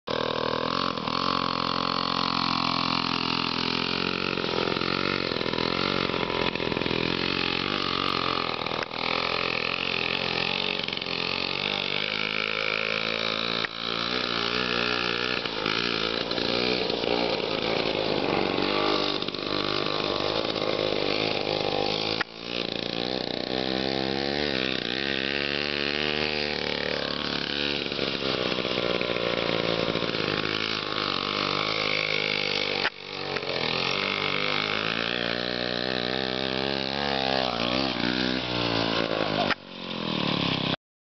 Power Products - PowerTrike, my motorized tricycle
Here's the final assembly. 1950's Power Products 2-cycle engine, MCM Tuff Trike, some creativity, custom red clearcoat paint, and disc brakes! Russ gets his 41 seconds of fame on the PowerTrike!! More vids to come.